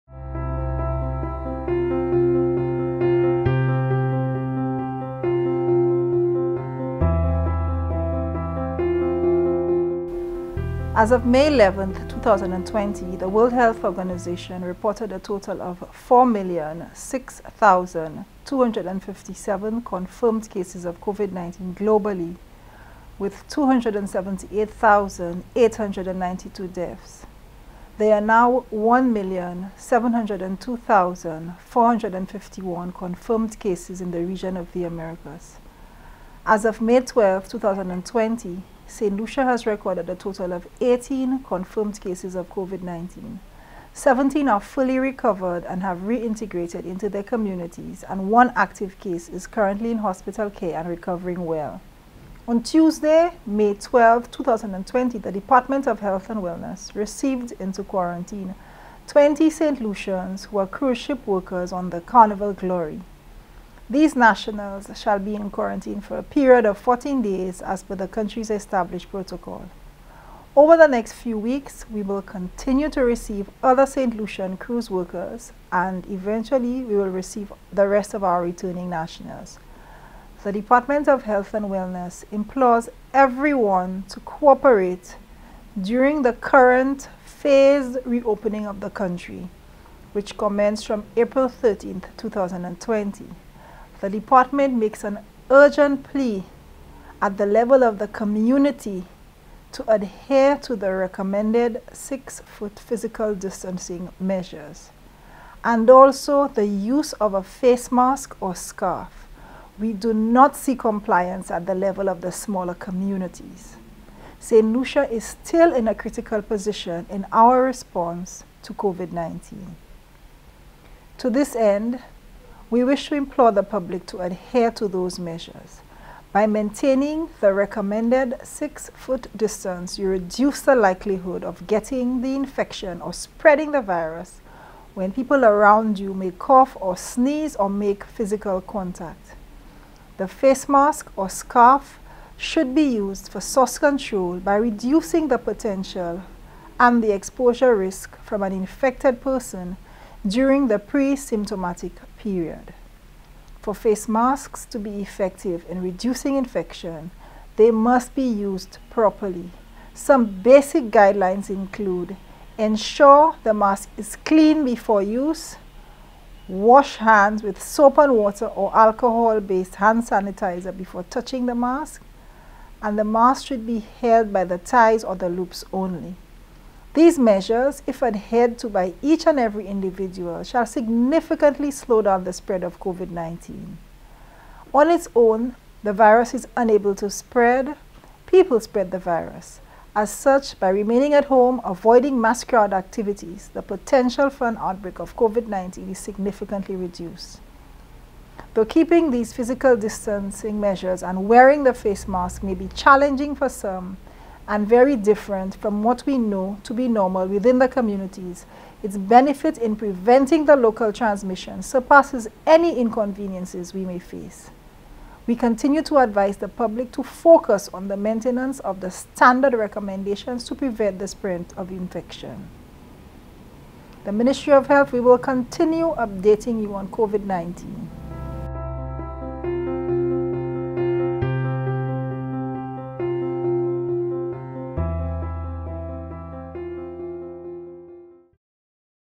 Adherence to COVID-19 measures within communities
Saint Lucia is still at a very critical position in the response to the national COVID-19 threat. To this end, the Ministry of Health urges community members to adhere to the recommended physical distancing measures of 6 feet, and the use of a face mask or scarf. The face mask or scarf should be used for source control by reducing the potential exposure risk from infected persons during the “pre-symptomatic” period.